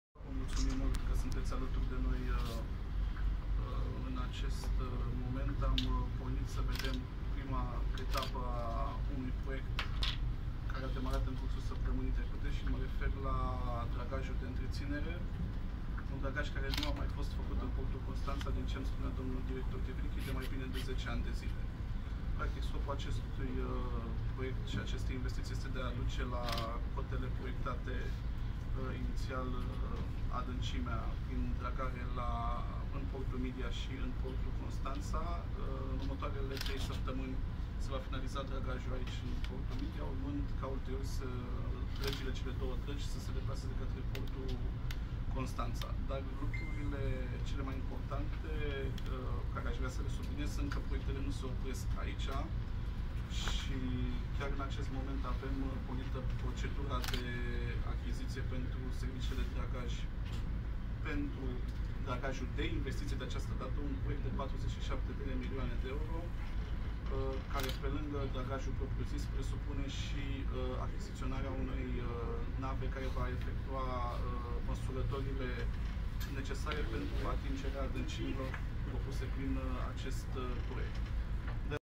ctnews.ro | ministrul Transporturilor, Razvan Alexandru Cuc despre investițiile în Portul Constanța
Însoțit de directorul interimar CNAPMC Nicolae Dan Tivilichi și președintele CJC  Horia Țuțuianu Razvan Alexandru Cuc s-a deplasat în Portul Midia.